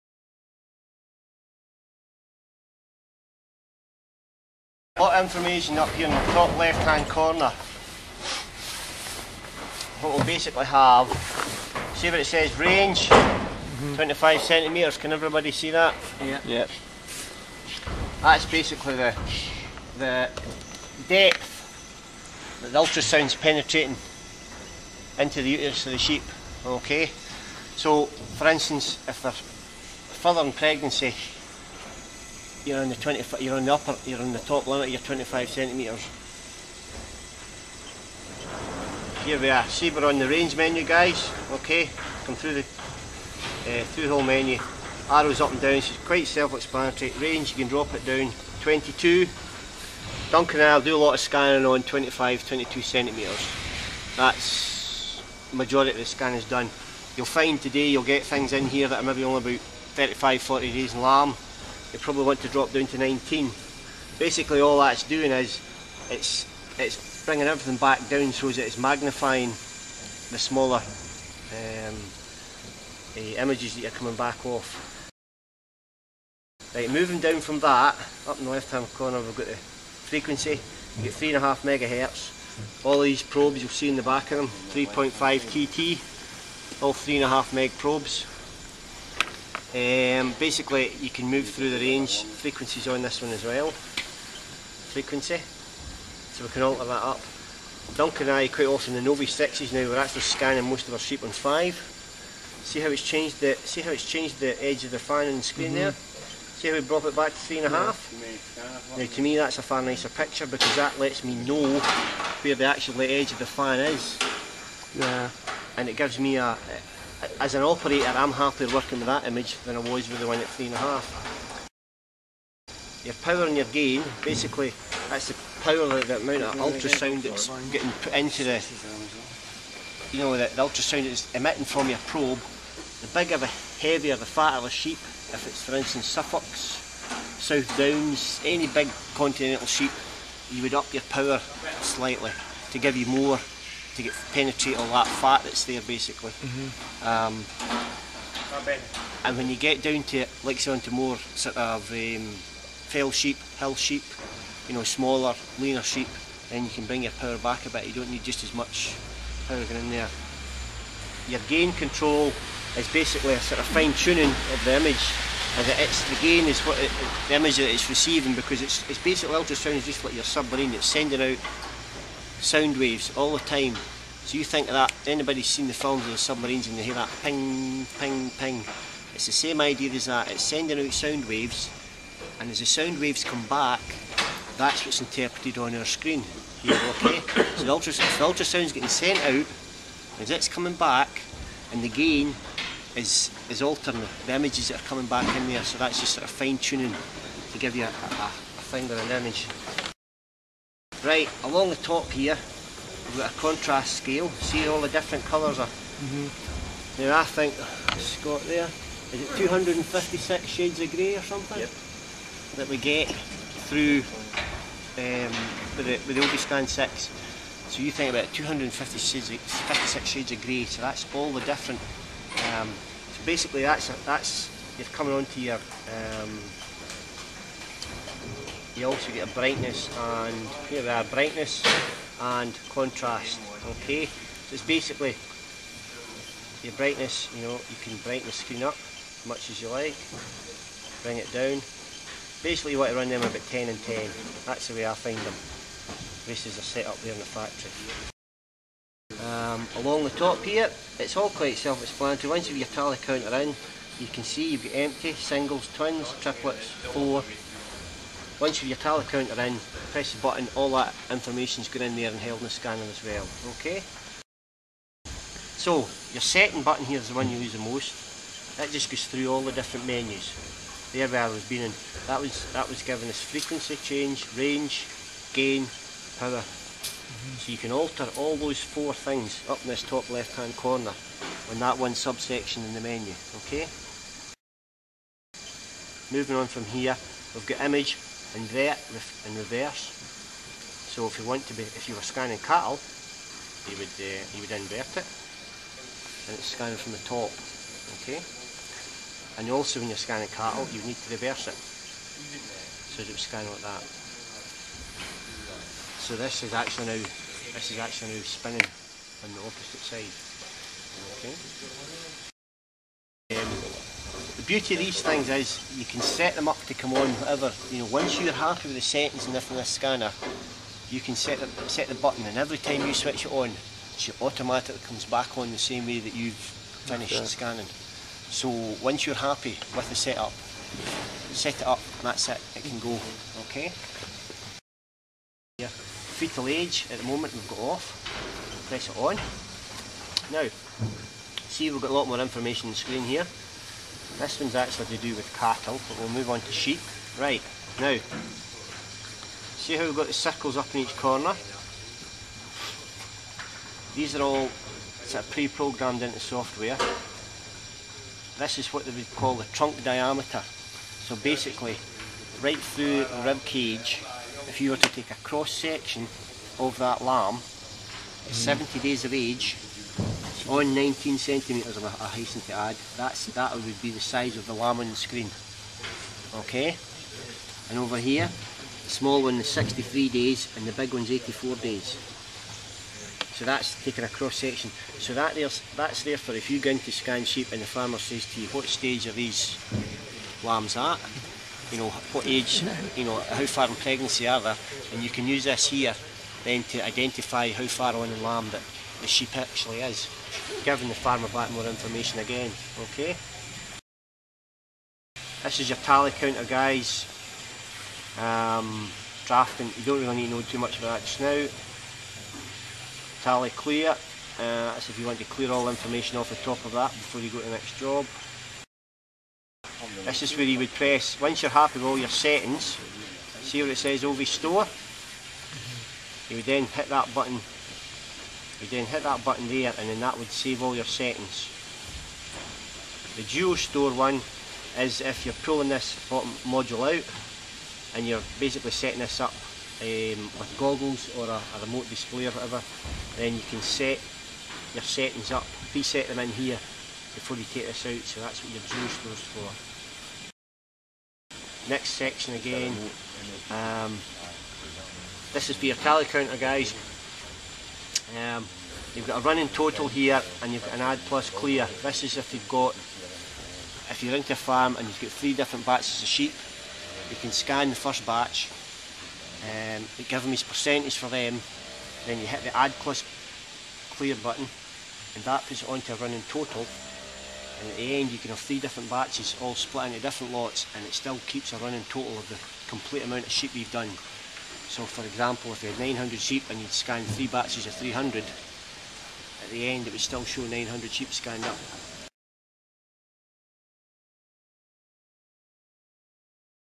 BCF Technology sheep scanning video 3 - Ovi-Scan settings explained
This is the third in a series of instructional videos explaining how to scan sheep for pregnancy. In this video, Bruce Gallacher, Sheep Scanning Instructor with Progressive Animal Services Ltd explains all the settings of the Ovi-Scan ultrasound scanner.

The video was shot at an IMV imaging sheep scanning training course (previously known as BCF Technology), run and organised by Progressive Animal Services Ltd. The trainers were Duncan Kennedy and Bruce Gallacher. Many thanks to both for their help with these videos.